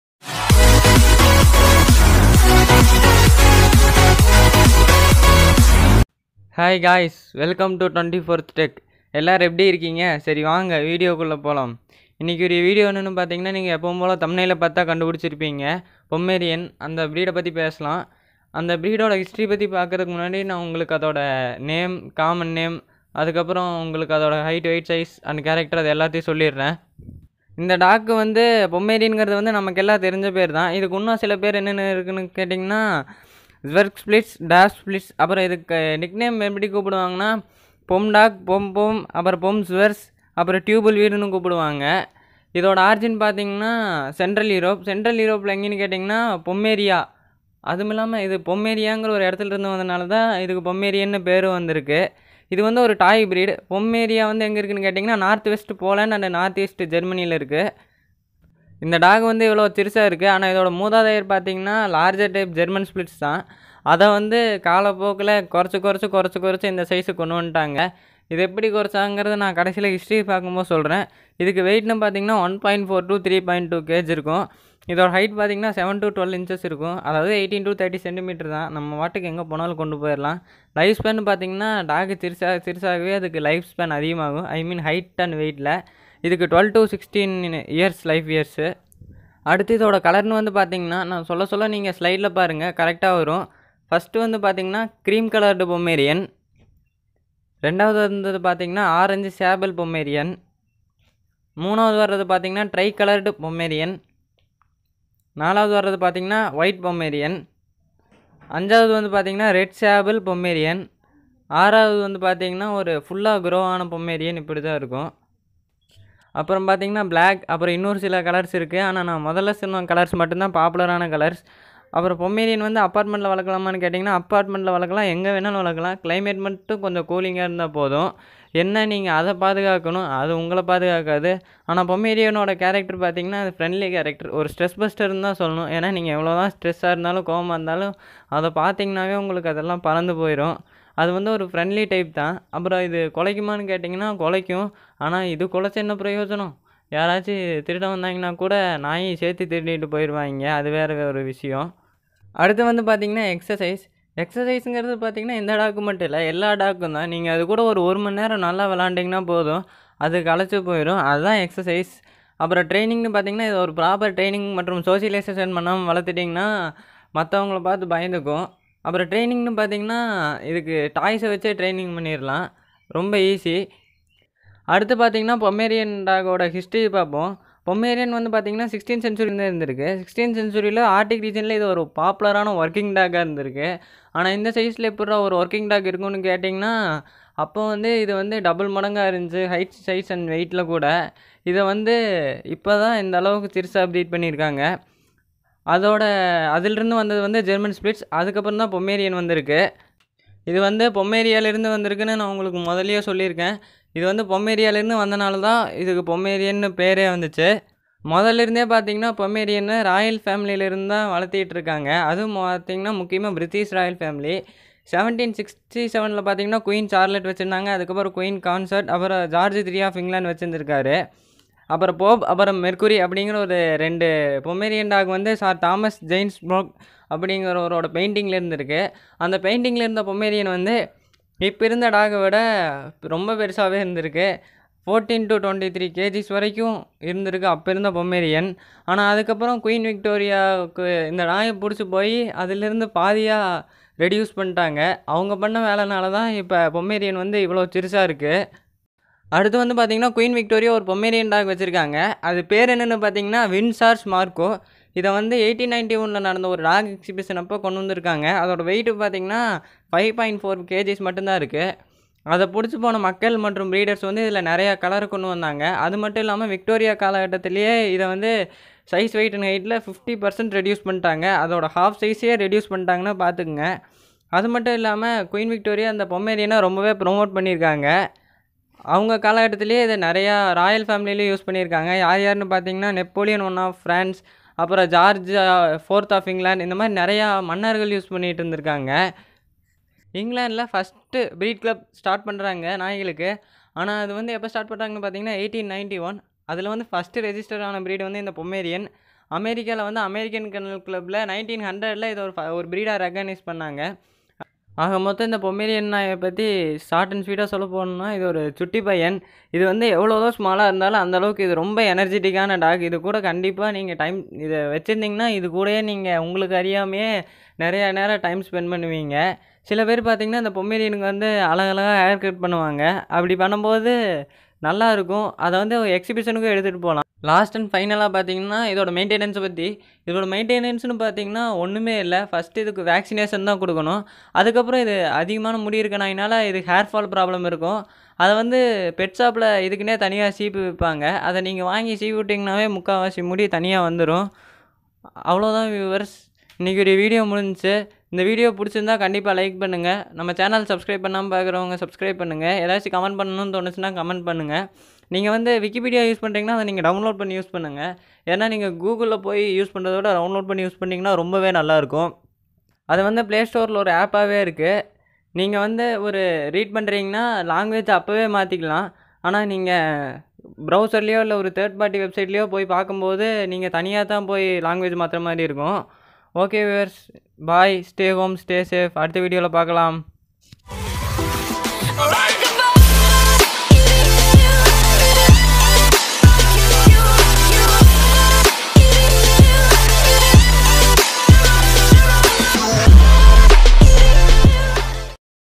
pomeranian நாய் பற்றிய முழு தகவல் | Full Information about pomeranian dog | In Tamil | 24th Tech
The Pomeranian (often known as a Pom) is a breed of dog of the Spitz type that is named for the Pomerania region in north-west Poland and north-east Germany in Central Europe. Classed as a toy dog breed because of its small size, the Pomeranian is descended from larger Spitz-type dogs, specifically the German Spitz. It has been determined by the Fédération Cynologique Internationale to be part of the German Spitz breed; and in many countries, they are known as the Zwergspitz ("Dwarf-Spitz").